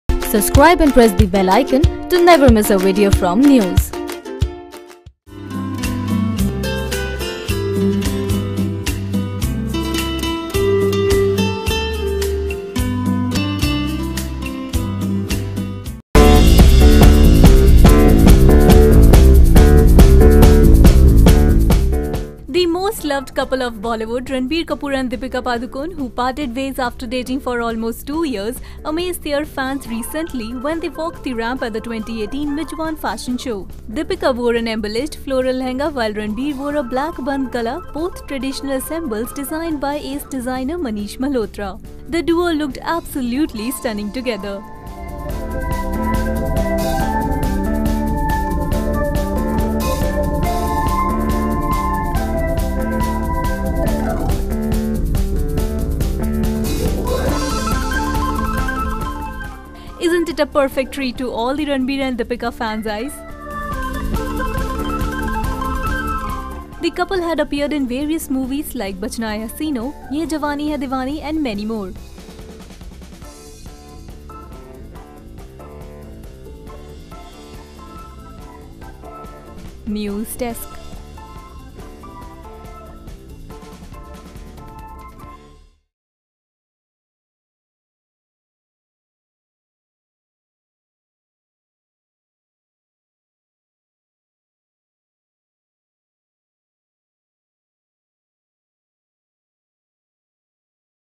Ex-lovers Ranbir Kapoor, Deepika Padukone walk the ramp hand-in-hand
The most loved couple of Bollywood, Ranbir Kapoor and Deepika Padukone who parted ways almost a year back amazed their fans, recently when they walked the ramp at the ‘2018 Mijwan Fashion Show’, wearing ensembles by designer Manish Malhotra.
Deepika wore an embellished floral lehenga while Ranbir wore a black bandhgala - both traditional ensembles designed by Malhotra.
The duo looked absolutely stunning together.
Isn’t it a perfect treat to Ranbir-Deepika fans?
The couple appeared in various movies like ‘Bachna Ae Haseeno’, ‘Yeh Jawaani Hai Deewani’ and many more.